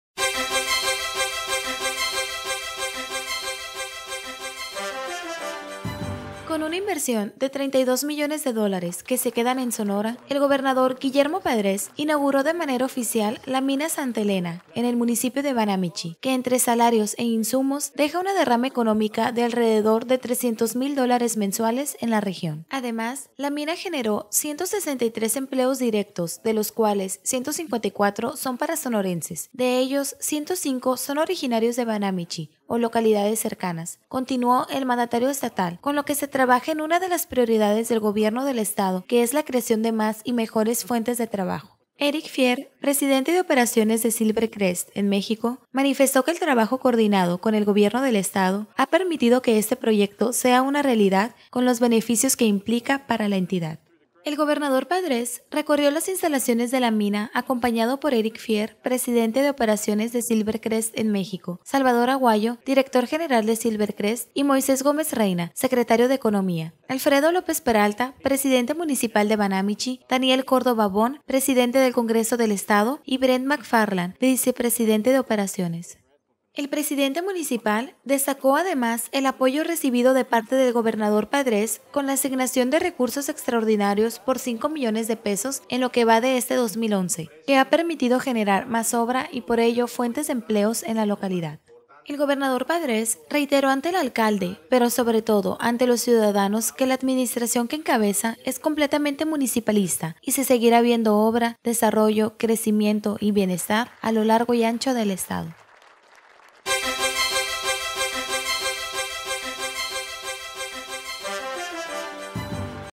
Gobernador Guillermo Padrés inauguró de manera oficial la mina Santa Elena. 20-05-2011
Banámichi, Sonora. Con una inversión de 32 millones de dólares que se quedan en Sonora, el Gobernador Guillermo Padrés inauguró de manera oficial la Mina Santa Elena, en el municipio de Banámichi, que entre salarios e insumos deja una derrama económica de alrededor de 300 mil dólares mensuales en la región.

Fecha del evento: 20 de Mayo de 2011